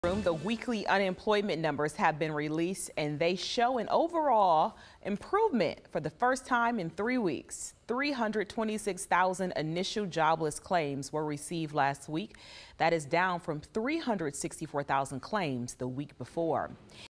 Unemployment numbers show decrease nationwide
326,000 initial jobless claims were received last week.